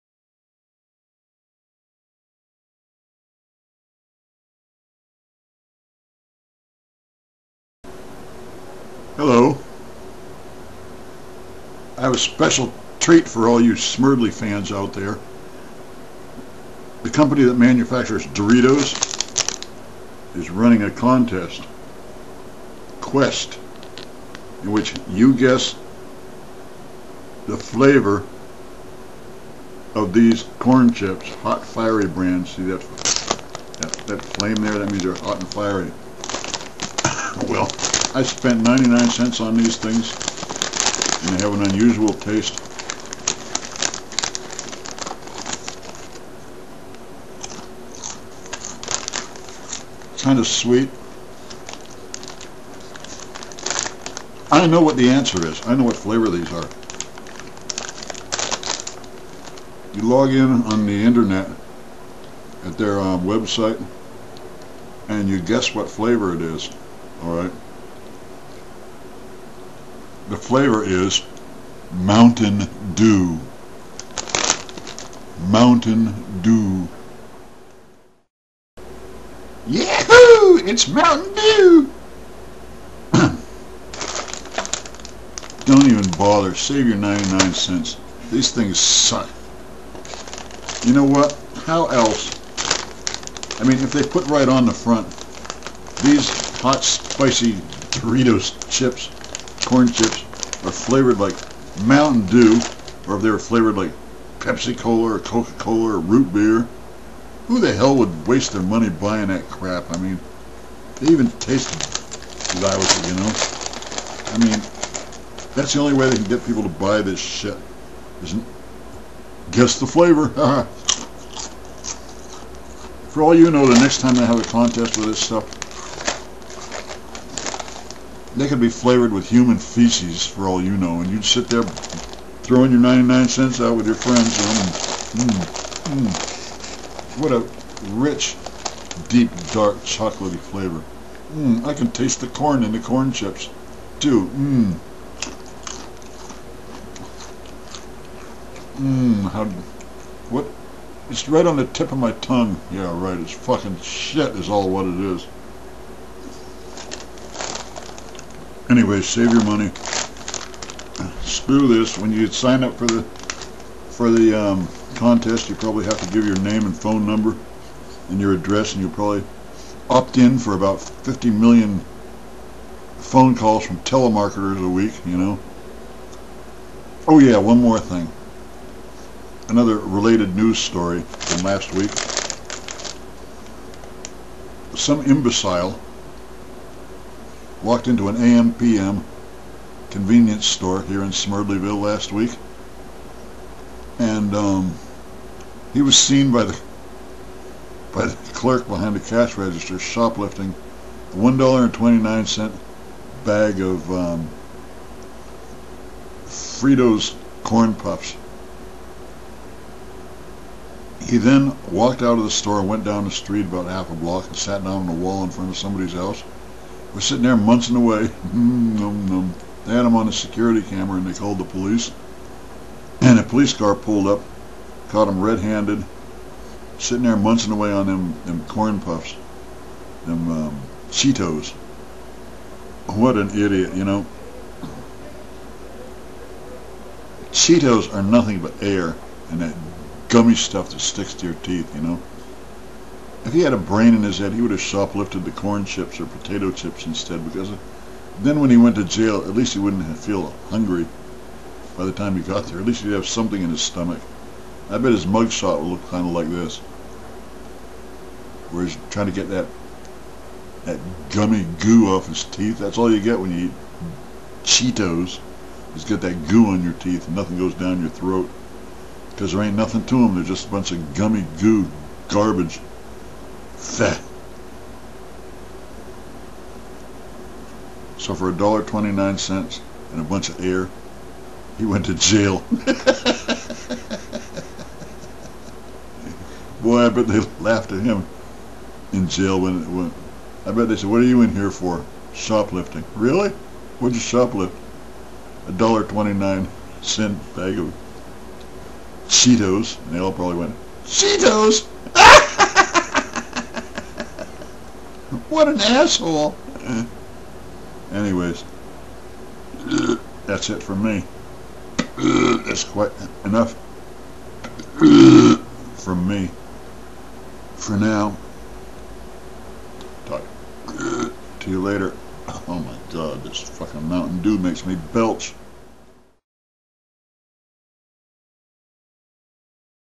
WIN The Doritos GUESS THE FLAVOR Contest
I have the correct answer!